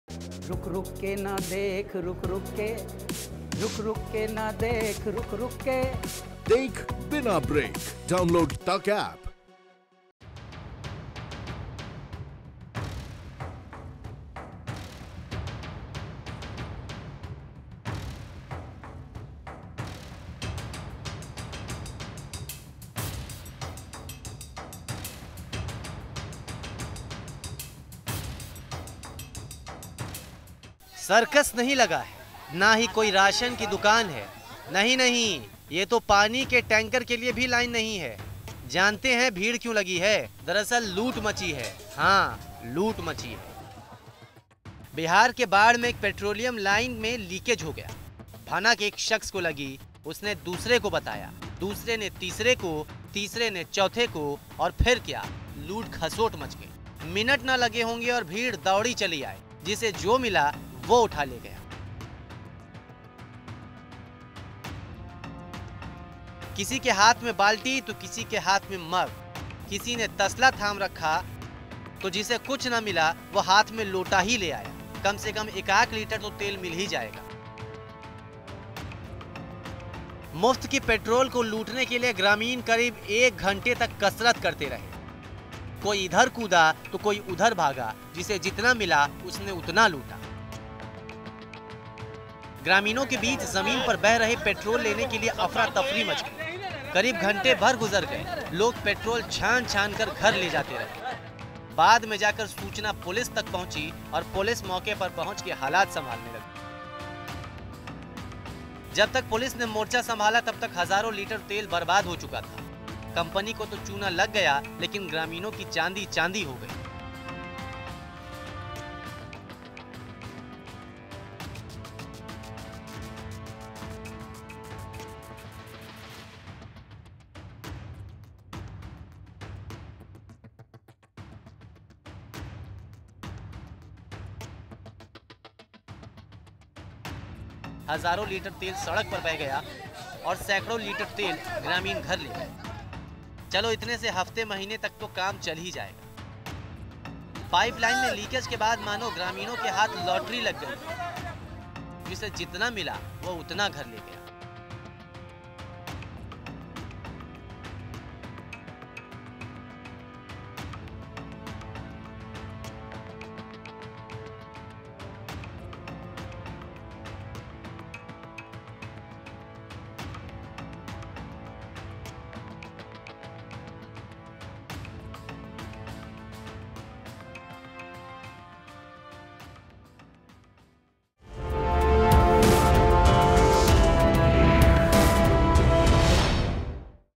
सड़क पर फ्री में तेल मिलता देख ग्रामीणों ने ये जुगाड़ निकाला ! NEWSTAK
पटना के बाढ़ में बरौनी तेल पाइपलाइन में रिसाव के बाद पेट्रोल लूटने के लिए लोगों में होड़ मच गई। तेल रिसाव की जानकारी मिलते ही लोग अपने घरों से बर्तन लेकर पेट्रोल लूटने पहुंच गए। पाइपलाइन कर्मियों ने तुरंत प्रशासन के मामले की जानकारी दी। पुलिस के पहुंचते ही मौके पर भगदड़ मच गई। पुलिस ने सभी लोगों को वहां से भगाया। कर्मचारियों ने कड़ी मशक्कत बाद रिसाव को बंद किया। पाइपलाइन कर्मियों ने आशंका जताई है कि किसी ने लाइन के साथ छेड़छाड़ की है। पुलिस पूरे मामले की छानबीन कर रही है।